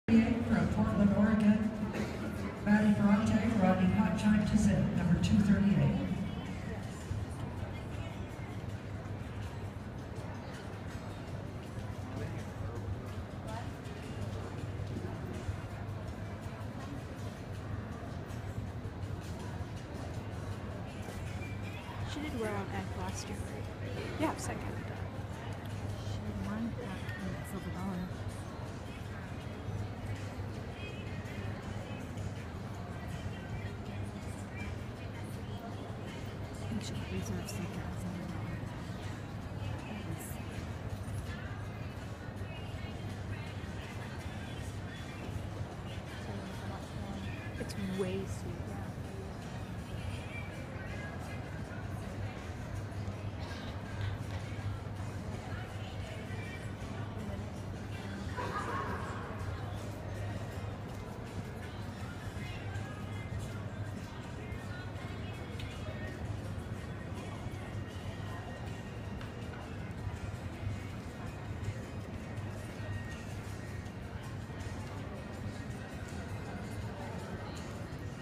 Hot Time To Zip equitation
Winning equitation pattern at the 2016 Novice Championship West show.